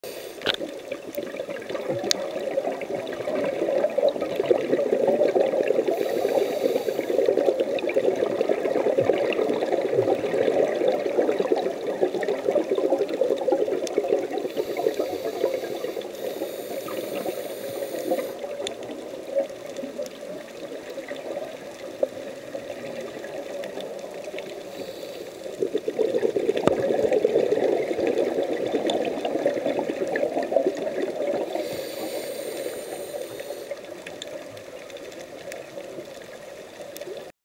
Synapta maculata
Holoturie répondant au doux nom de "Ceinture Mauresque" en train de se nourrir sur un fond sableux.  Oman, Province de Musamdam, Lima Rock, avec Nomad Ocean Adventures le 07/05/2010.